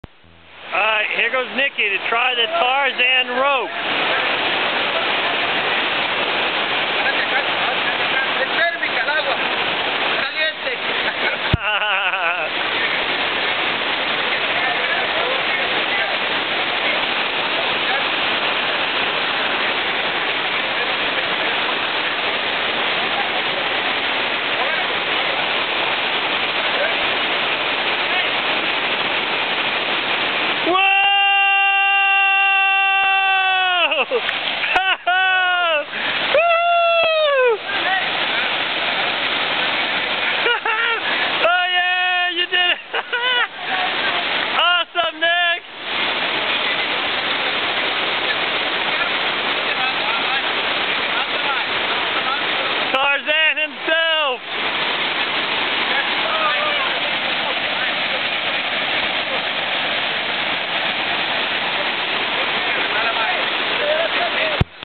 Tarzan Swing into Fortuna Waterfall
January 2010: Nick is getting instructions from guide on how to hold the rope